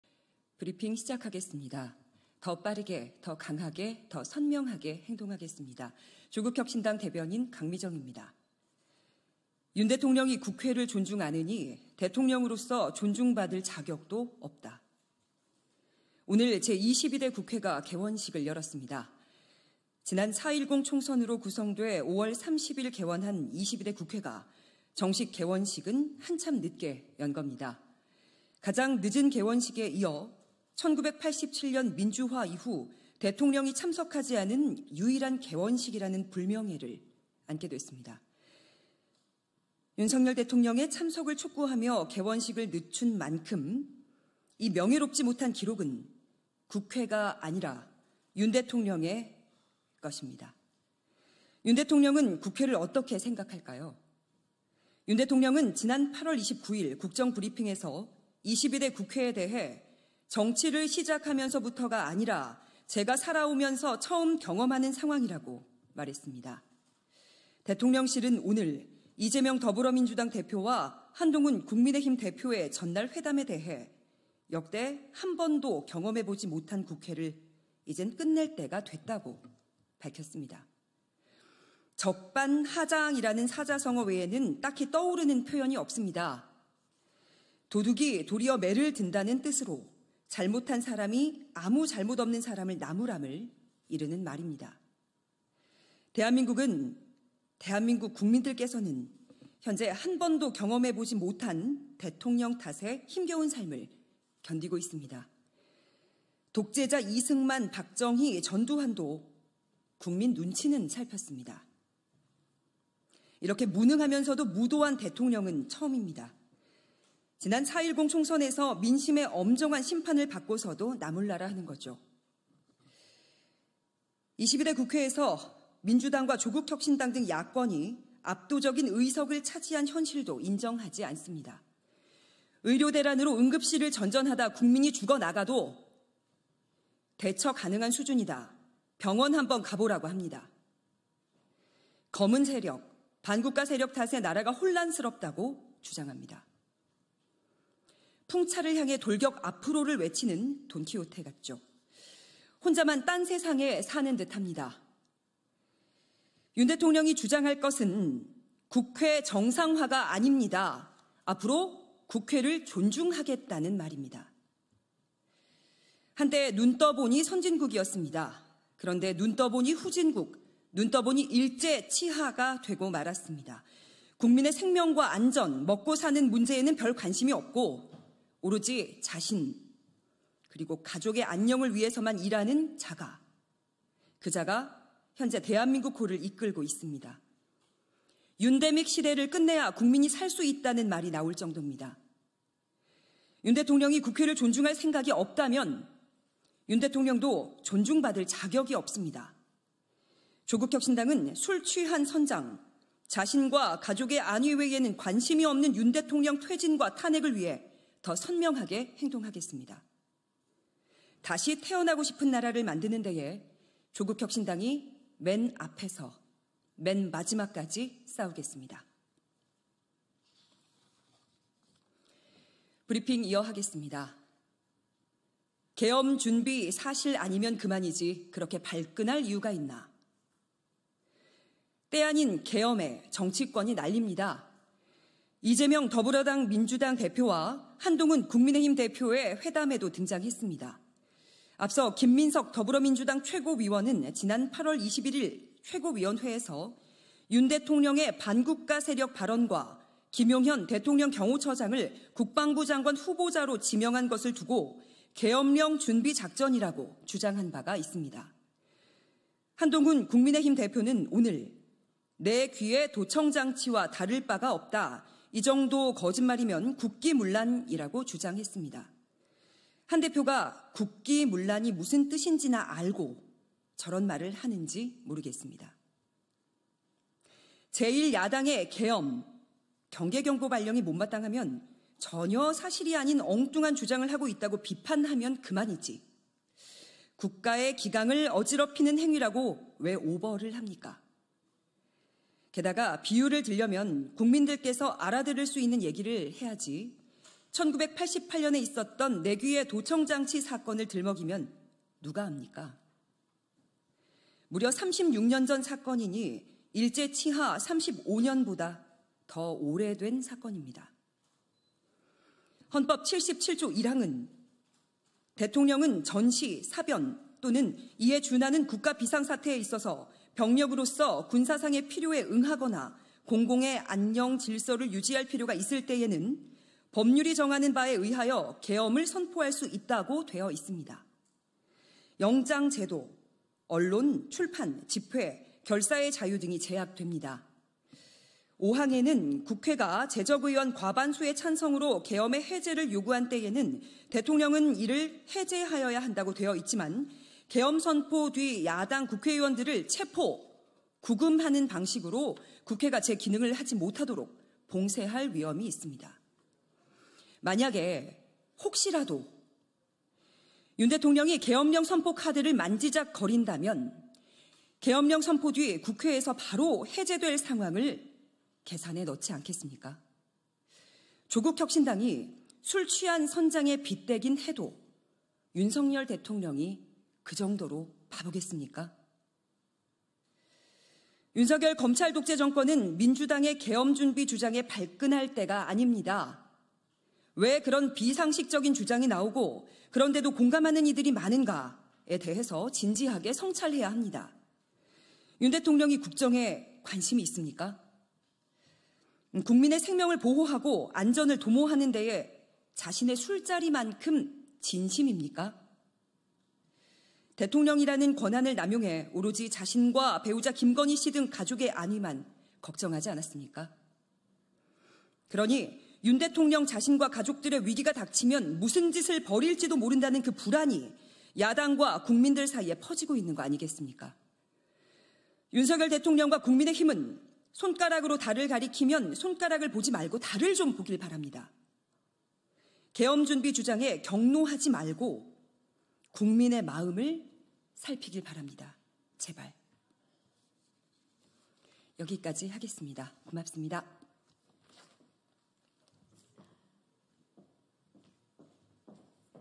대변인님 "윤대통령 국회 존중 않으니 대통령으로서 존중받을 자격 없다.!"
강미정 대변인님 응원 또 응원합니다